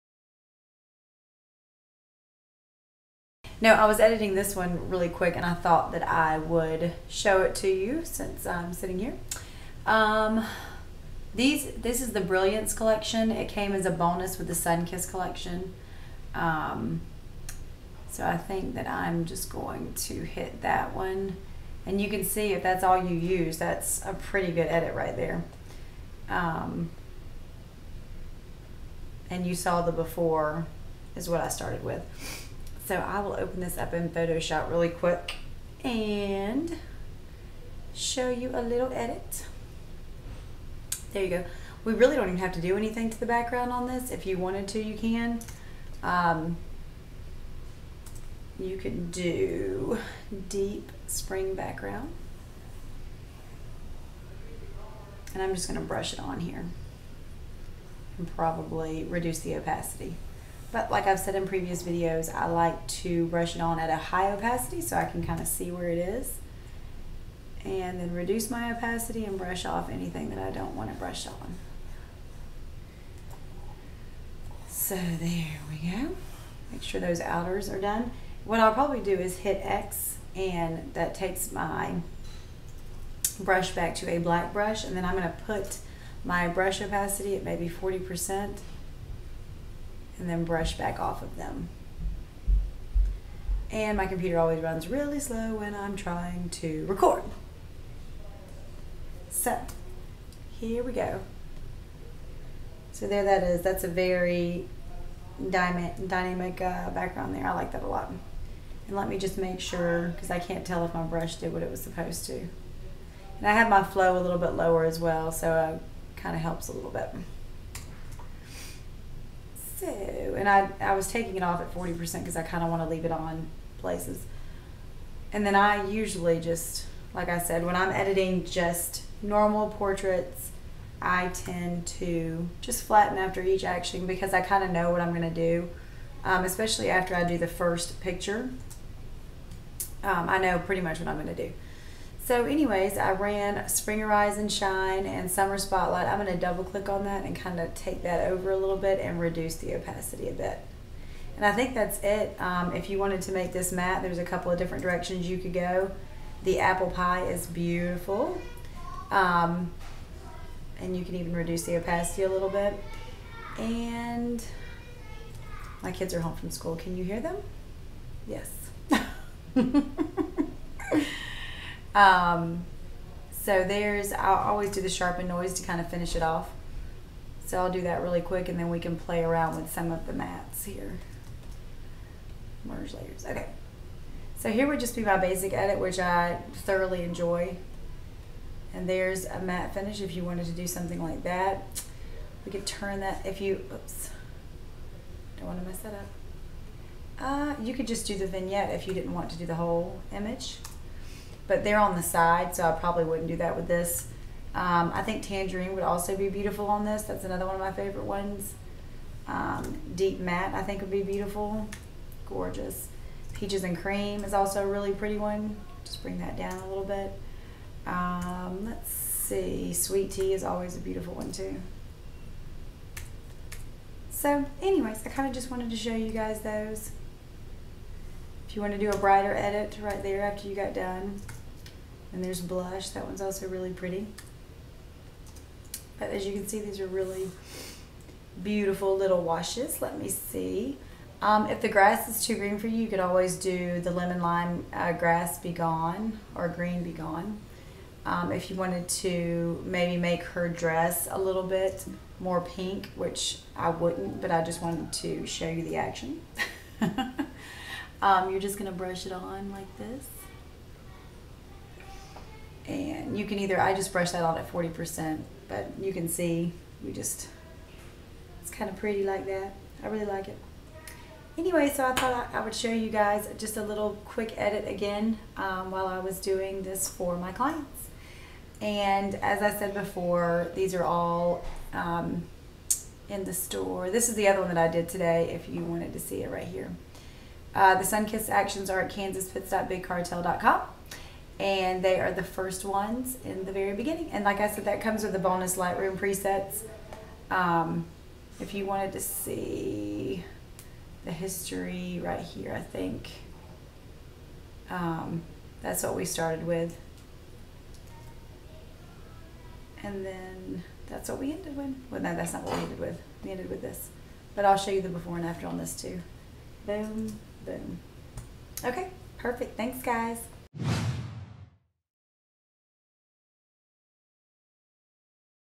Editing Yummy Light Sibling Pictures with the Sunkissed Collection from Kansas Pitts Photography
Want to bring out that gorgeous summer light in your images? Yeah it doesn't take long, just a few clicks really!